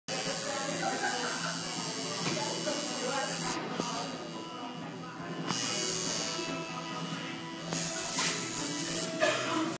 Getting tatto from Darren Brass!
It's so crazy to get a tatto from Darren brass in Miami ink